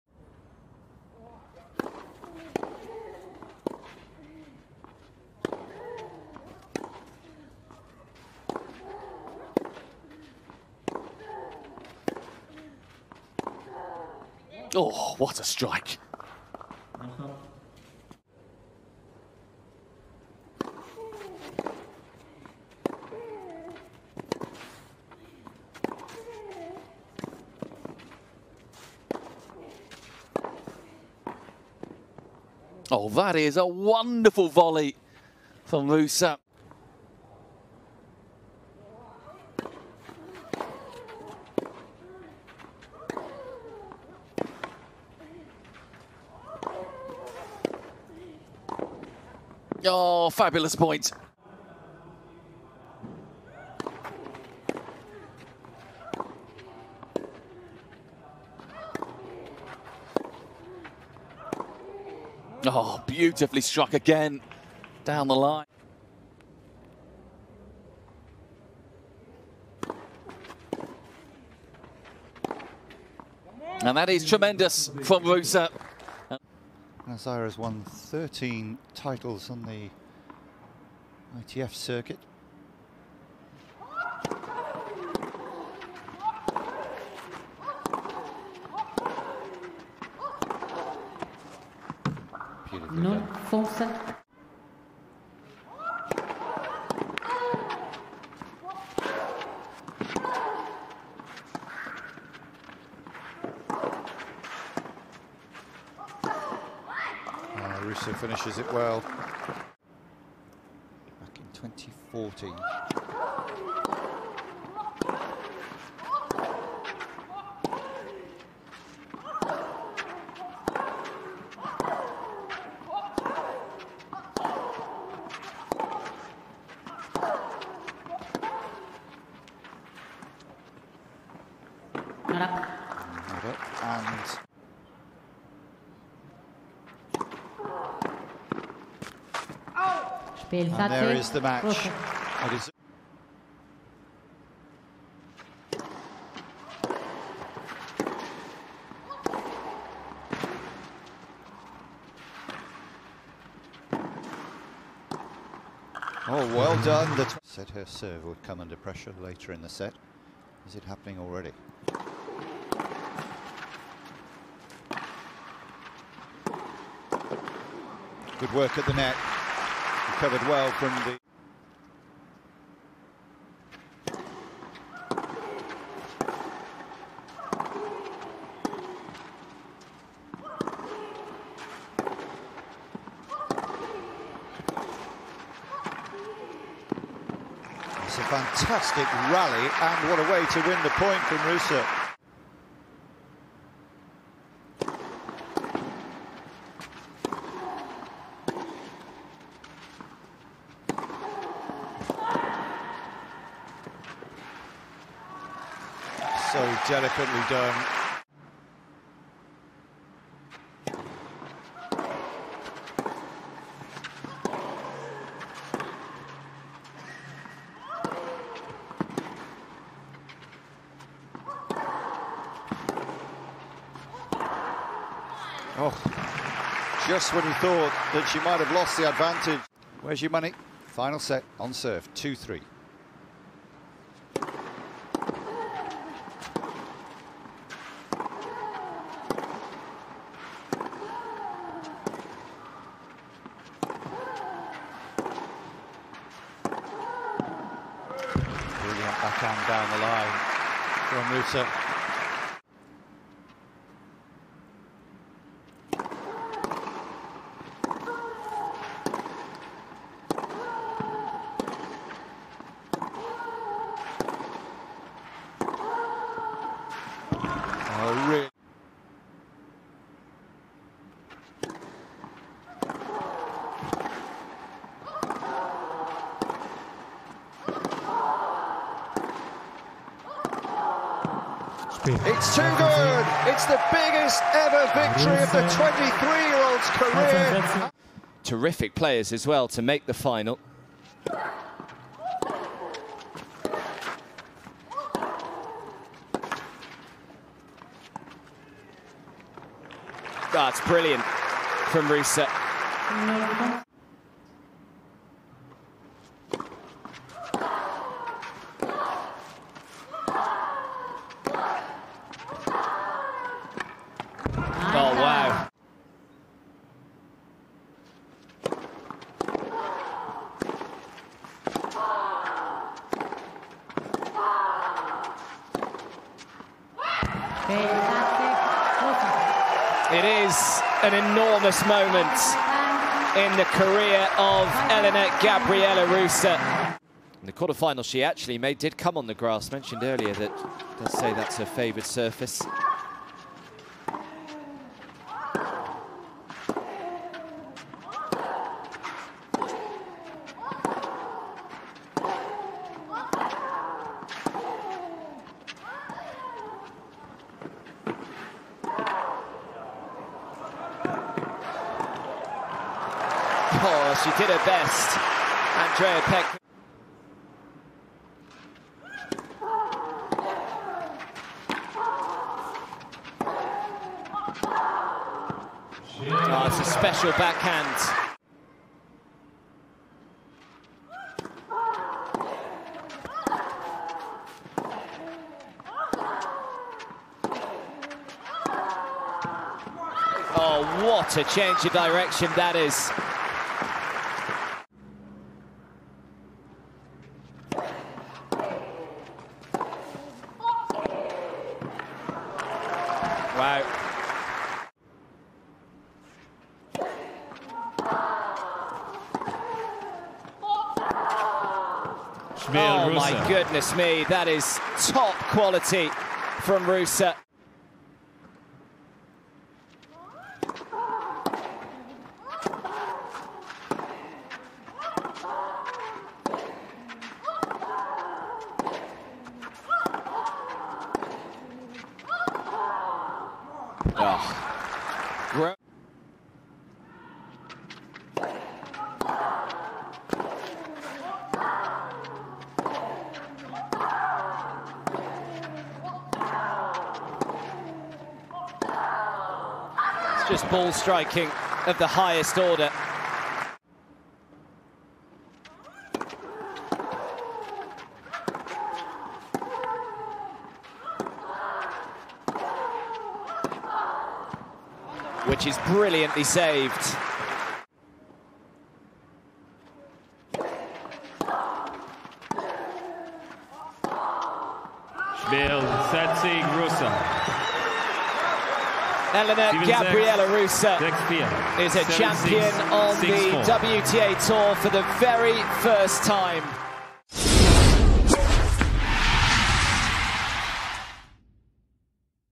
First-time WTA title winner Elena-Gabriela Ruse's BEST plays at Hamburg 2021 Tennis!
Relive the best points throughout the winning run by 2021 Hamburg European Open champion Elena-Gabriela Ruse, who lifted her first WTA title!